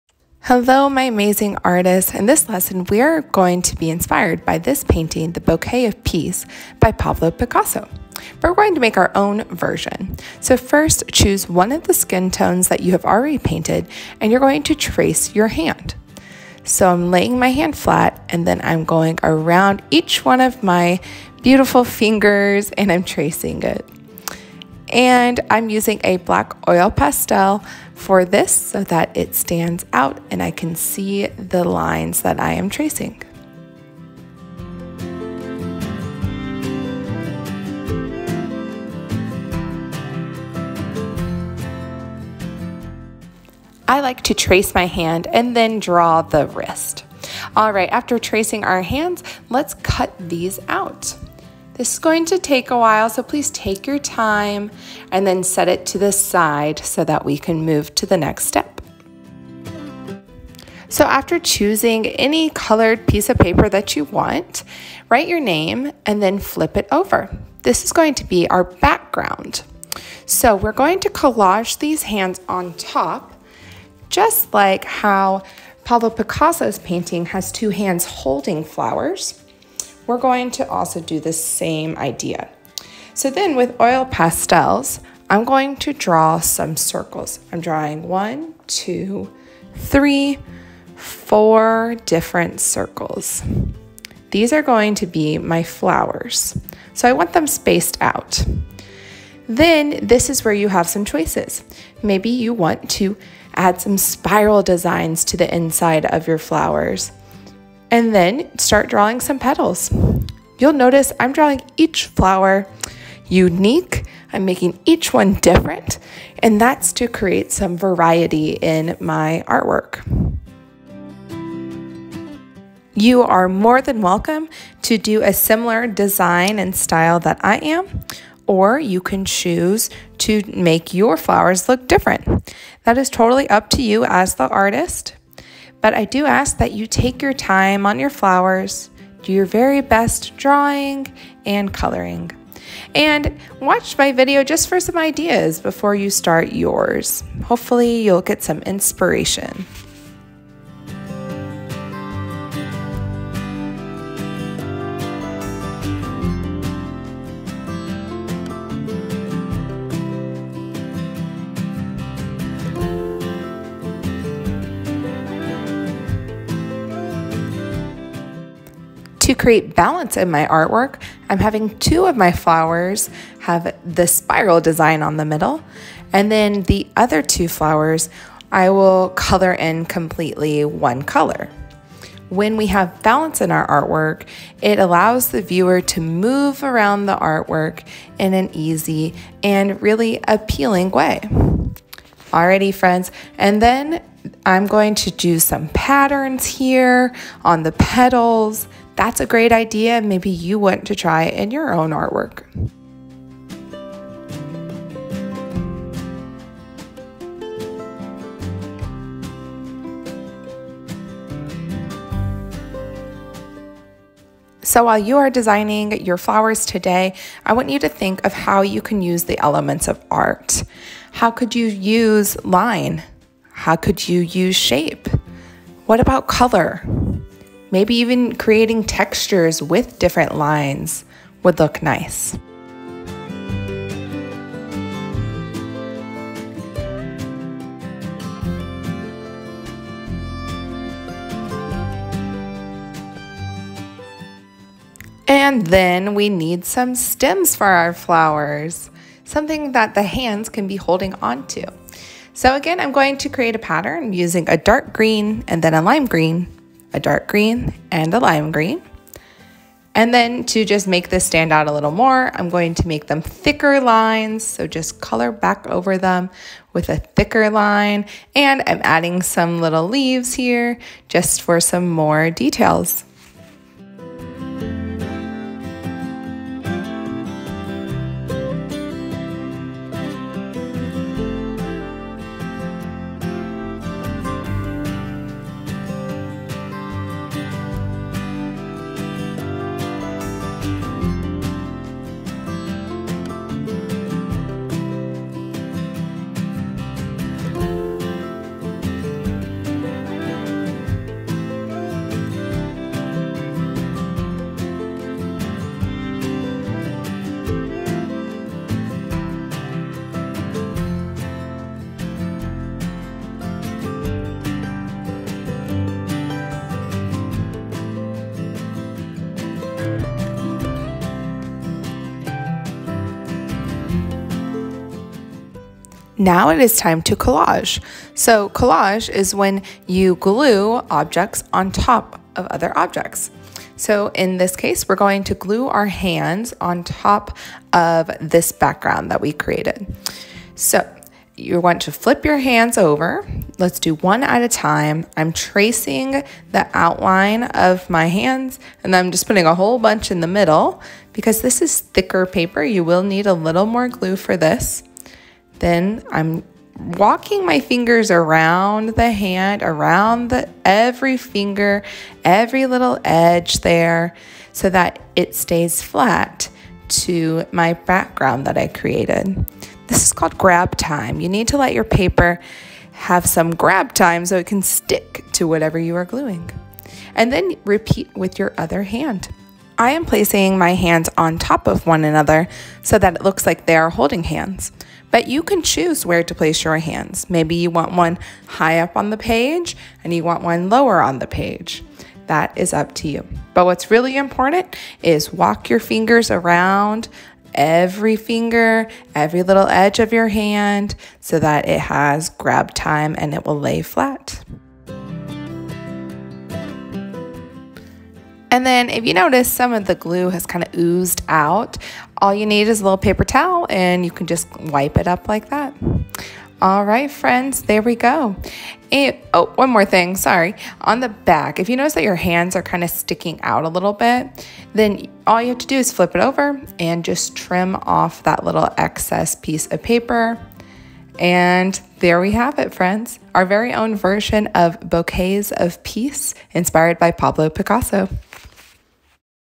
Peace Flowers
In this video we will create a collage inspired by Pablo Picasso's painting, Bouquet of Peace.